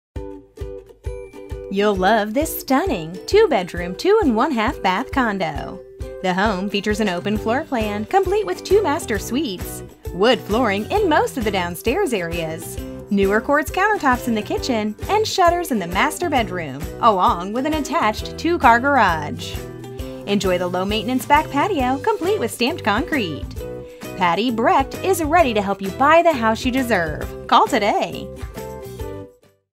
907 Marvin Gardens Way Rocklin, CA | ColdwellBankerHomes.com
907 Marvin Gardens Way, Rocklin, CA

You will love this stunning 2 bedroom, 2 1/2 bath condo located in the upscale community of Park Place located in the heart of Rocklin. The home features an open floor plan complete with two master suites, wood flooring in the downstairs livable areas, newer quartz countertops in the kitchen, shutte